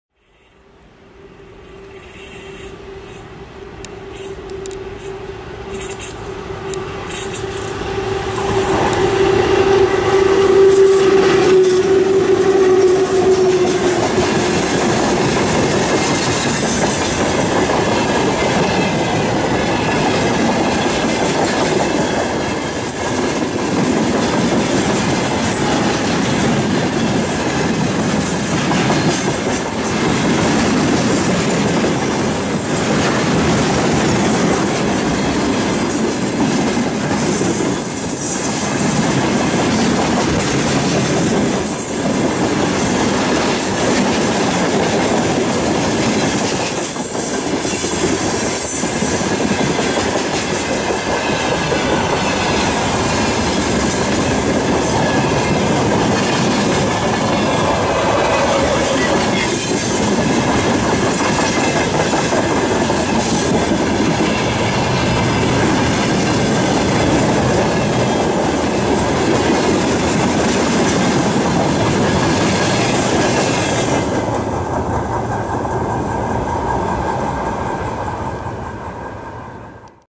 Электровоз ВЛ10У-553 с грузовым поездом
Электровоз постоянного тока ВЛ10У-553 (приписан ТЧЭ-5 Орехово) с грузовым поездом, следует по 3 пути.

Снято на перегоне Царицыно - Красный Строитель, участок станция Царицыно - платформа Покровское, Курское направление пути, Московская ж/д. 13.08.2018 г.